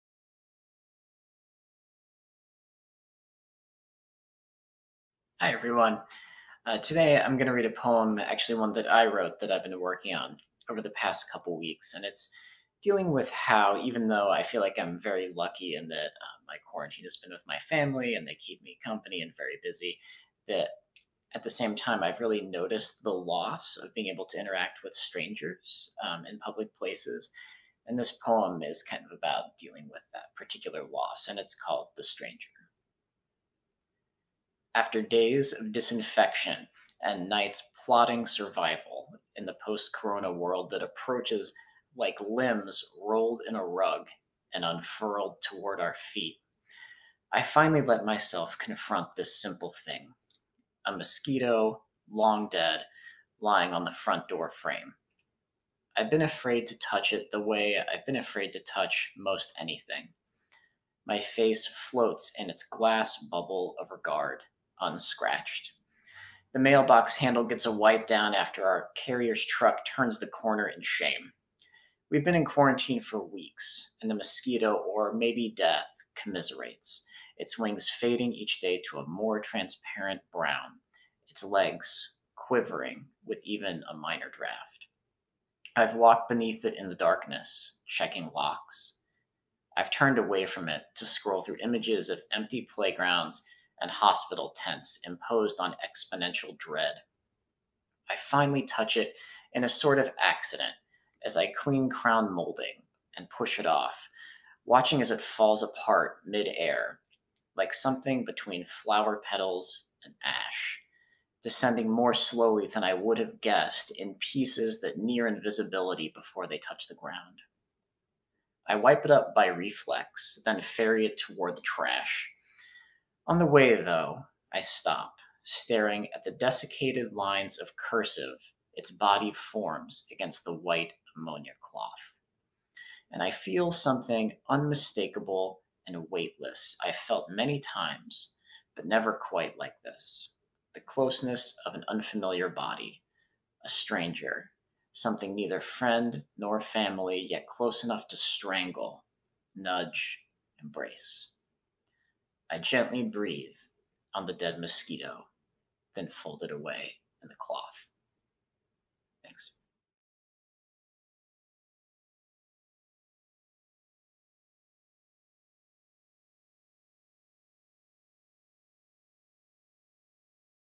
Matthew Sumpter - National Poetry Month 2020
Celebrate National Poetry Month with the Tulane Libraries!

Video submission by Matthew Sumpter for National Poetry Month 2020 reading an original poem titled "The Stranger."

Video edited by Alan Velasquez